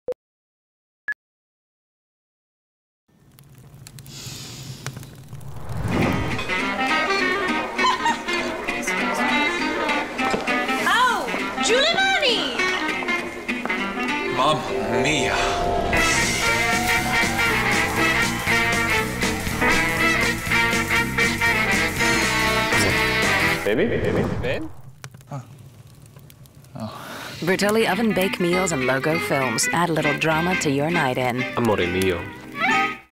Bertolli Pasta & Logo Films
Me (the waiter) in the Bertolli commercial, aired Feb & March 2009.